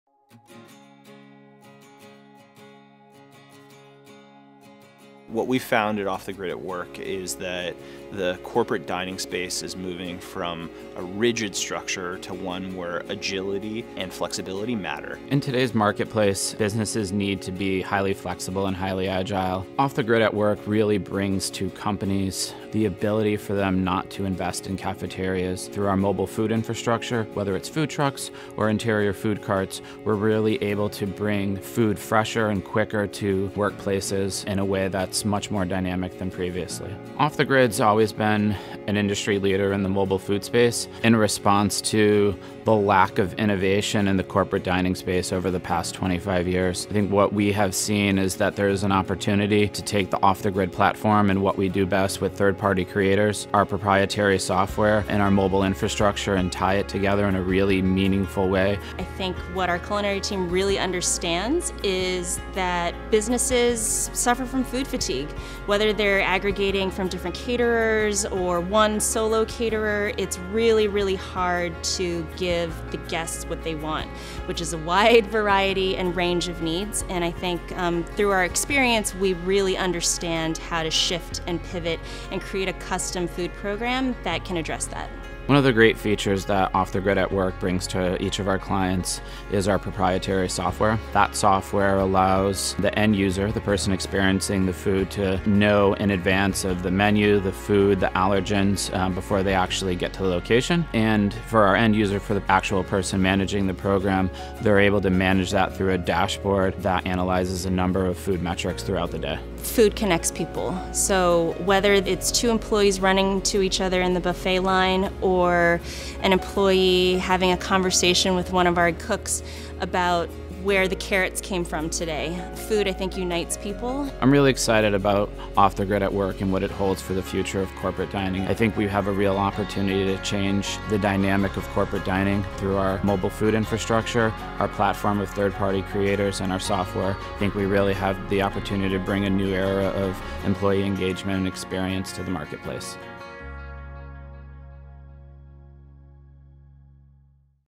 Off the Grid at Work
Off the Grid at Work is a scalable employee dining program rooted in enhancing each meal through flexible, mobile food services. As the only in-office solution that truly brings small businesses to corporate dining spaces, we believe in the power of a shared meal to build community and connection.